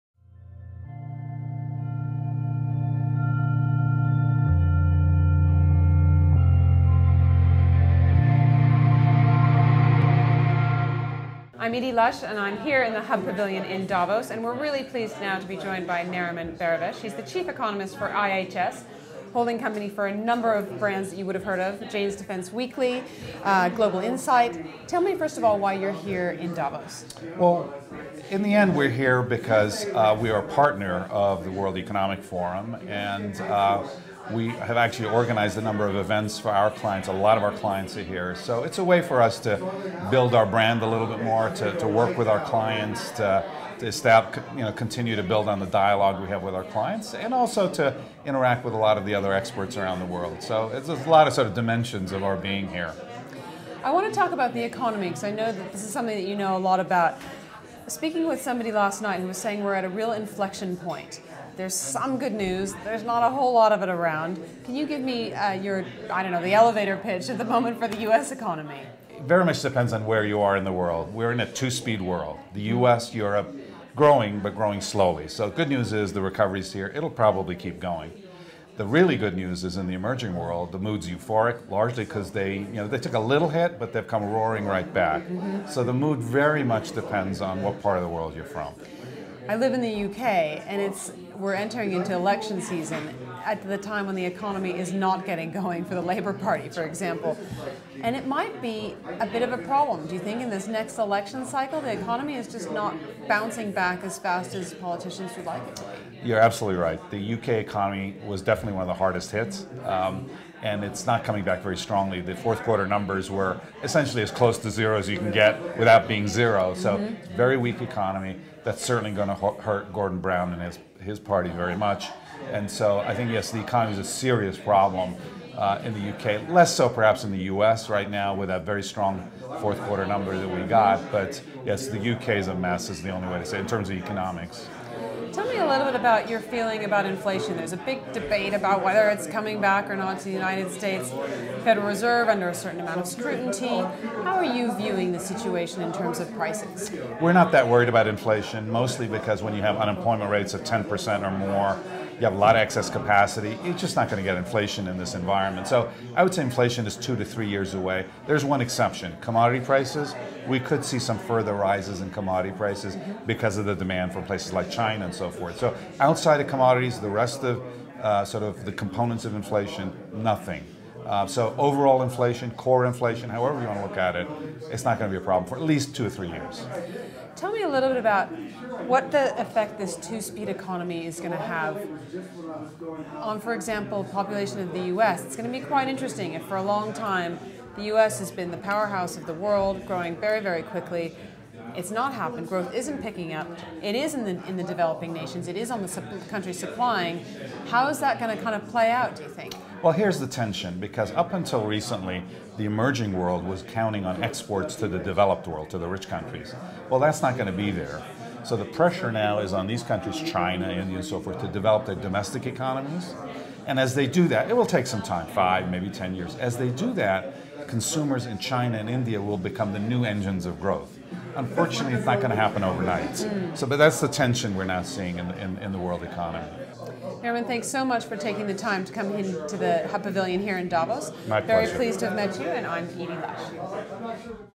Hub Culture Davos 2010: Nariman Behravesh - Chief Economist, IHS
Nariman Behravesh, the Chief Economist for IHS, tells Hub Culture's Edie Lush that there's some good news regarding the economy -- especially in the emerging world.  Filmed on location at the Hub Culture Pavilion in Davos.